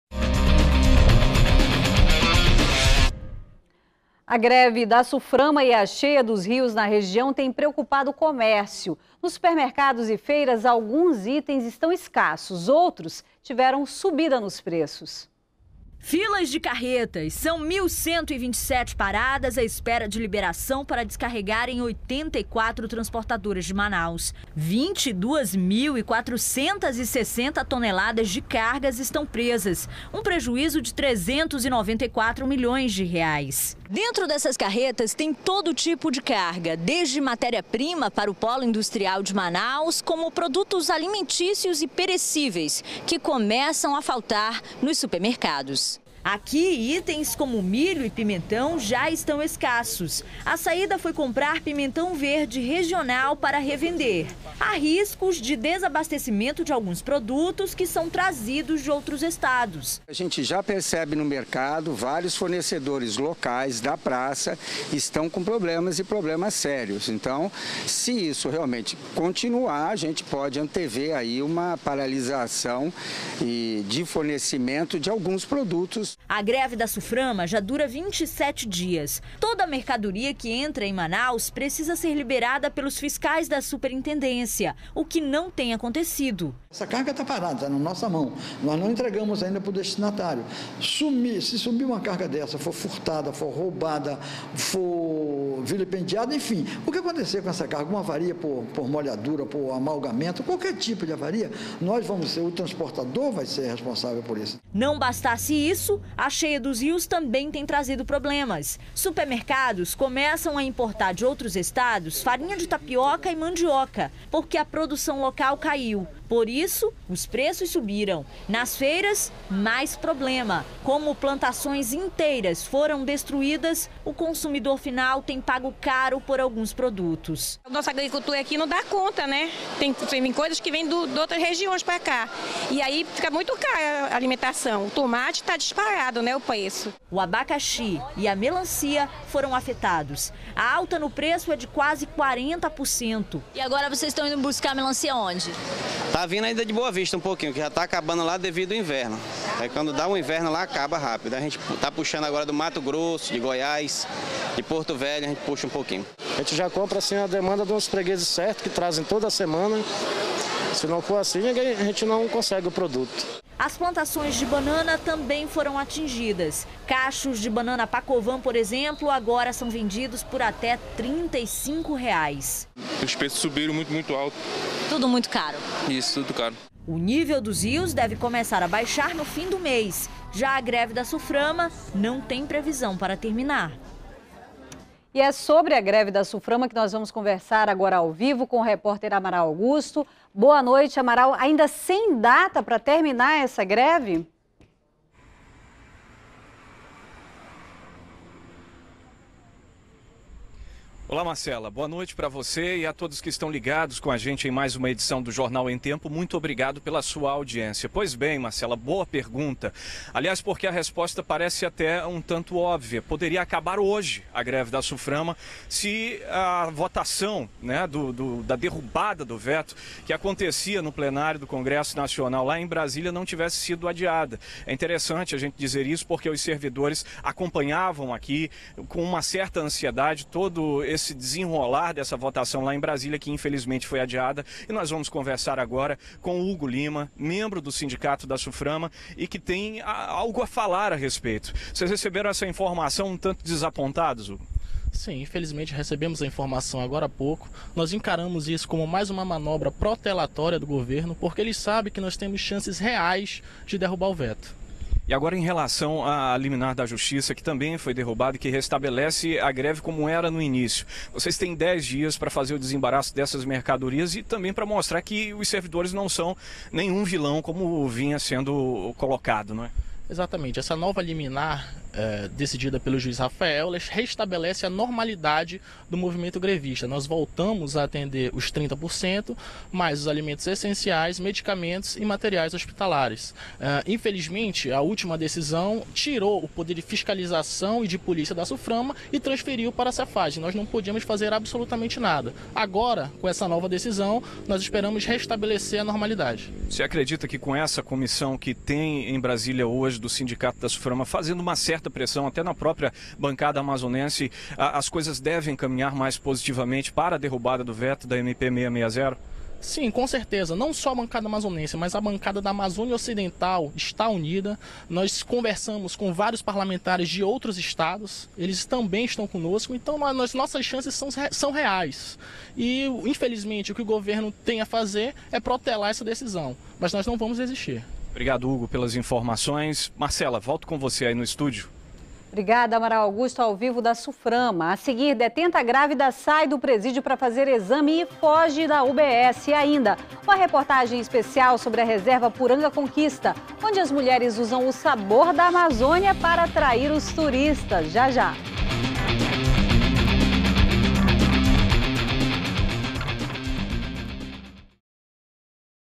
Greve da Suframa e cheia dos rios fazem faltar alguns itens em feiras e supermercados
02º BLOCO JORNAL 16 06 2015 - Greve da Suframa e cheia dos rios fazem faltar alguns itens em feiras e supermercados